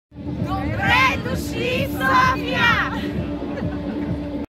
Welcome to Sofia
Welcome to Sofia! - from the Mayor of Sofia, Mrs.Yordanka Fandakova